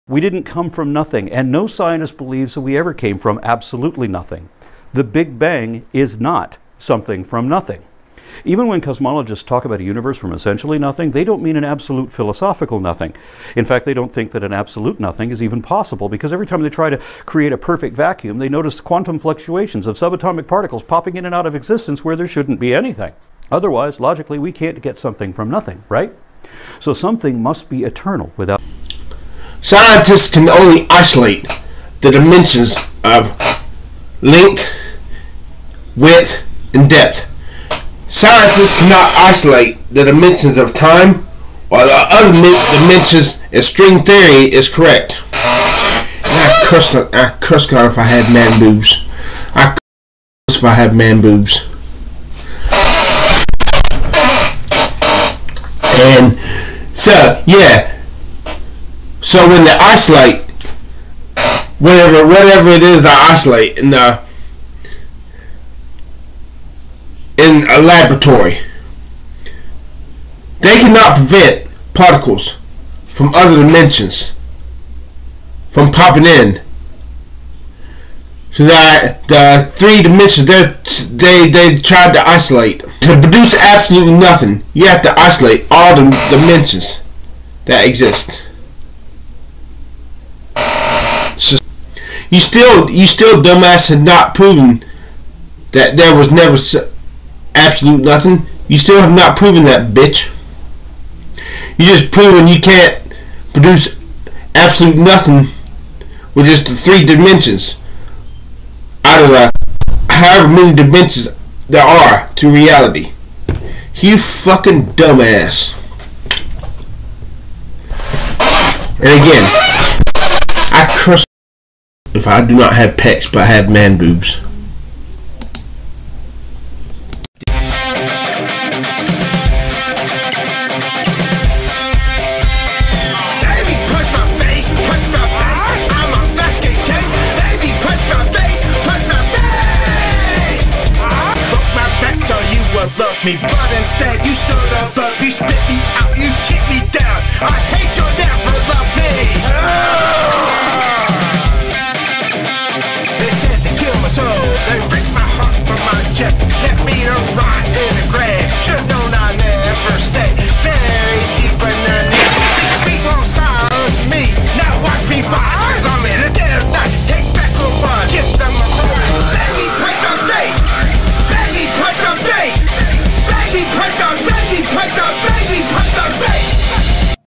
Aron Ra on what was before the Big Bang
Was there NOTHING before the Big Bang?  This is like saying you can go south at the South Pole